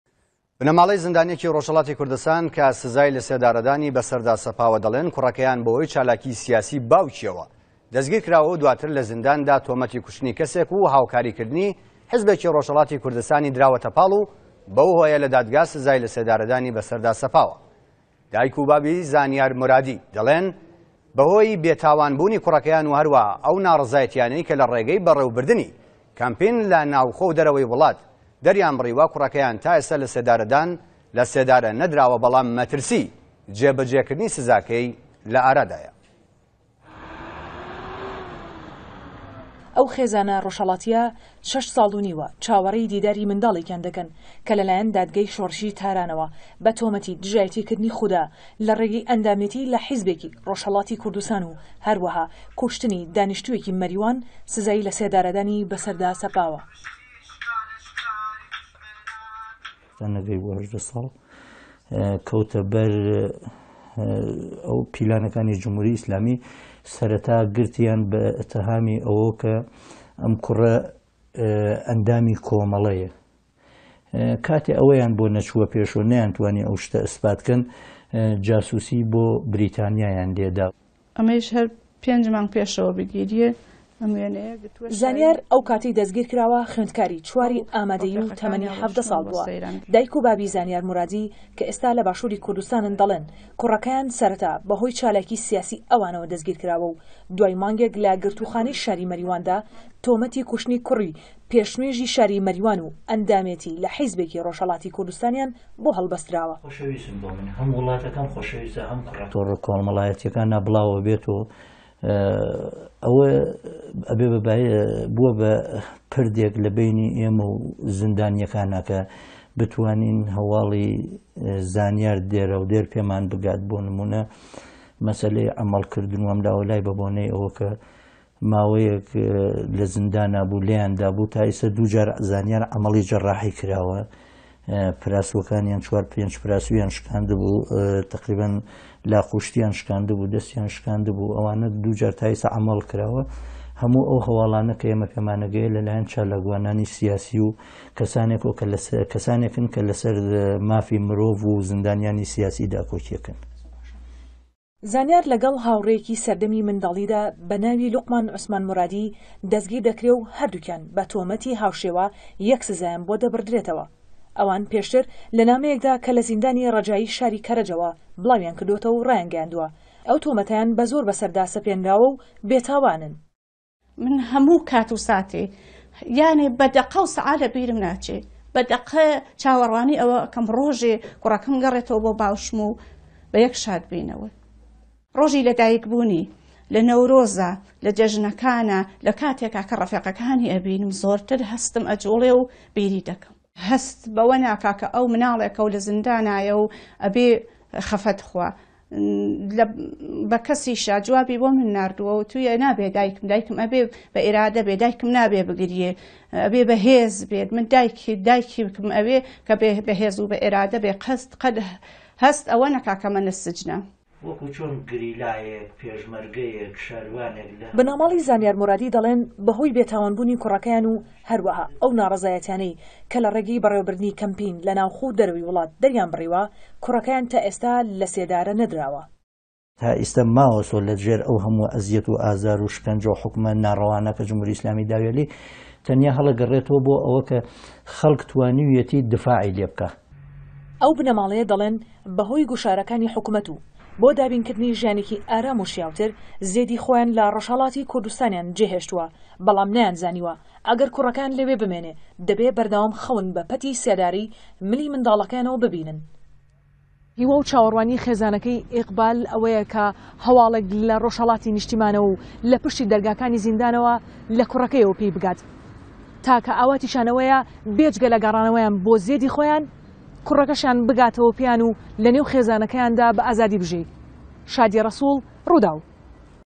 ئێقباڵ مورادی: هه‌م وڵاته‌که‌م خۆشه‌ویسته، هه‌م کوڕه‌که‌م...
وتووێژی که‌ناڵی رووداو له‌گه‌ڵ ئێقباڵ مورادی و ئامینه قادری باوک و دایکی زانیار مورادی زیندانی سیاسی مه‌حکووم به سێداره له زیندانی ره‌جایی شاری که‌ره‌ج